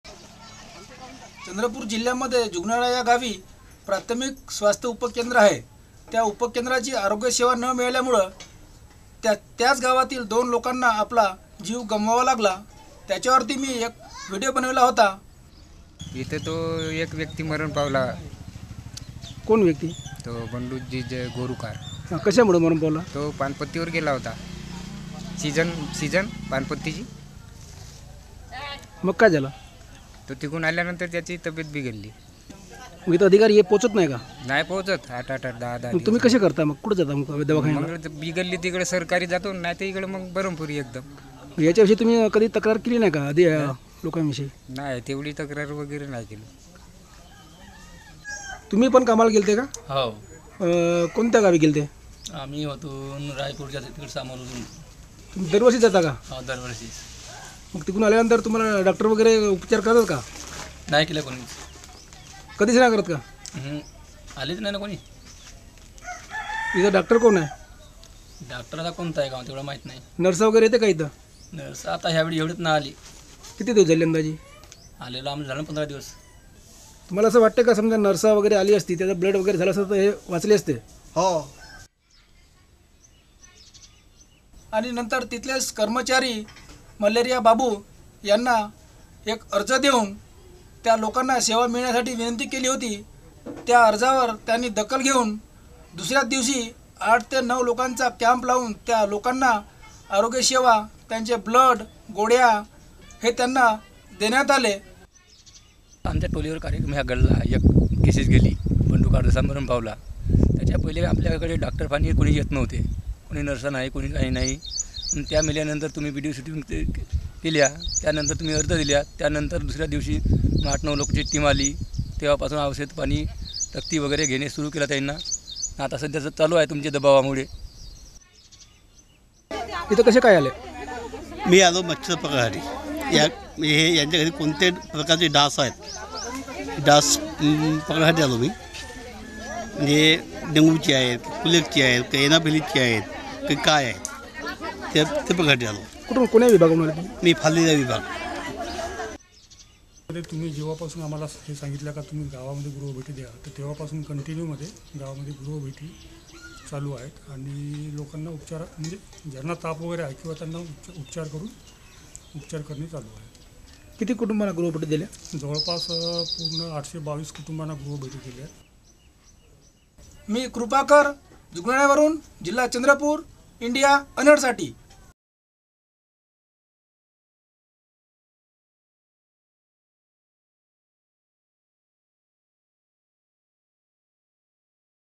Impact_Primary Health Center Providing Health Facilities_ Krupakar reports for IndiaUnheard
Health center started in Mangali village of Chandrapur district pf Maharashtra.

Community Correspondent Krupakar Chahande reports from Maharashtra for Video Volunteers.